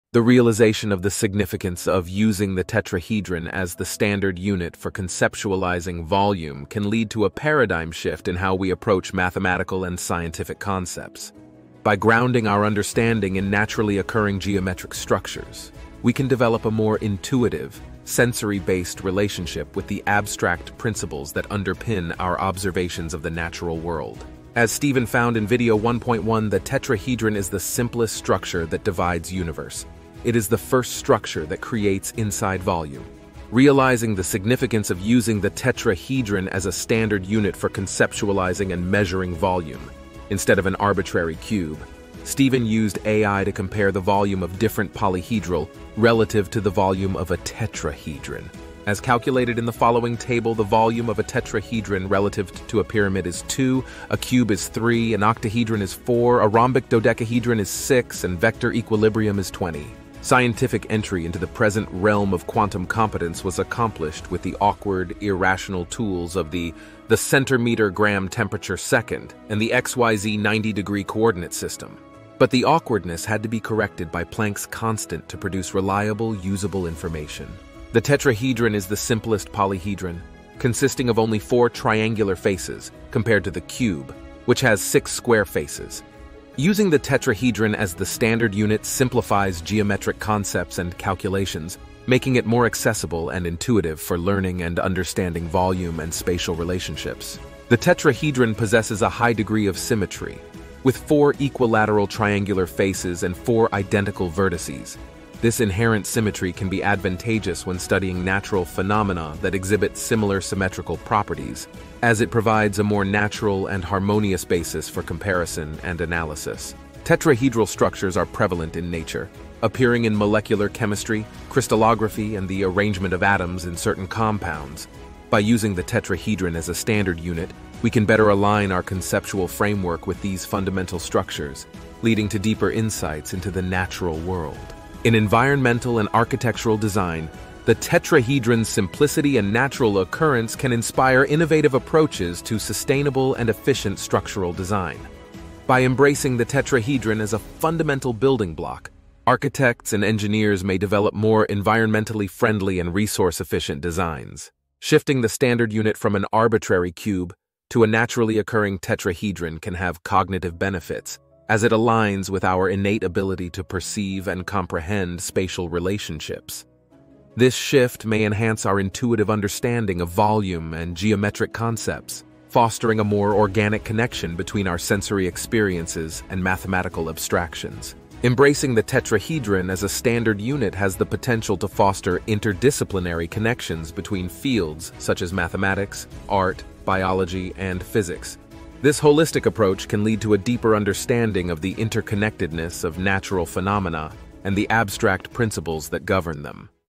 10.03 Using the tetrahedron as the standard unit of volume can change how we approach math & science
Realizing the importance of using the tetrahedron as the standard unit for understanding volume can change how we approach math and science. By relating abstract principles to natural geometric shapes, we can develop a more intuitive understanding. In video 1.1, Steven discovered that the tetrahedron is the simplest structure that divides the universe and creates inside volume. Recognizing this importance, Steven used AI to compare the volume of various polyhedral relative to the volume of a tetrahedron, rather than an arbitrary cube.

Tetrahedron vs Cube as a Unit of Volume

                                   Tetra         Cube
                                  volume 1 volume 1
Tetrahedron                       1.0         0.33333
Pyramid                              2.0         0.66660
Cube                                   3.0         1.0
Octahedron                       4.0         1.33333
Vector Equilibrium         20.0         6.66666